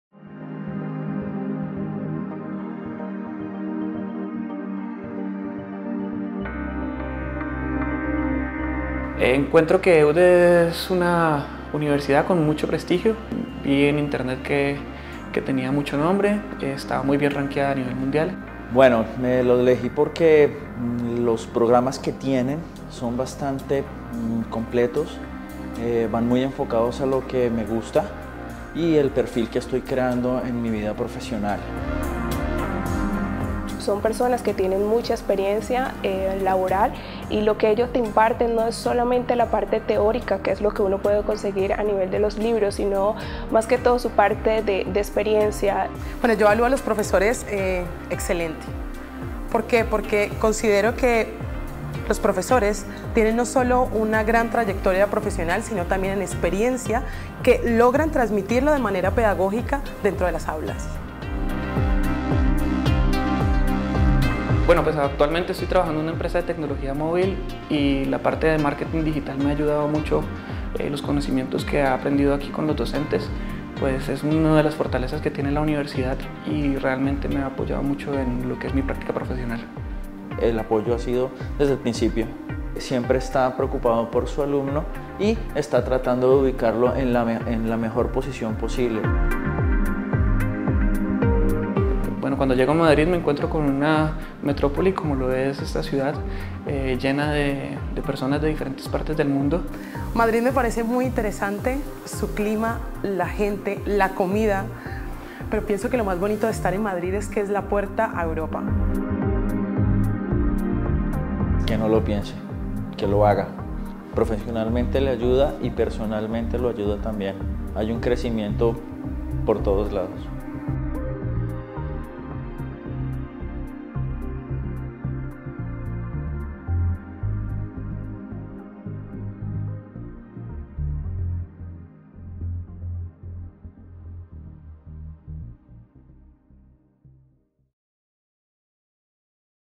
¿Qué opinan los alumnos colombianos de EUDE?
Nuestros alumnos procedentes de Colombia, no dudan en destacar algunas ventajas y cualidades de nuestra Escuela de Negocios. ¡Conoce sus opiniones!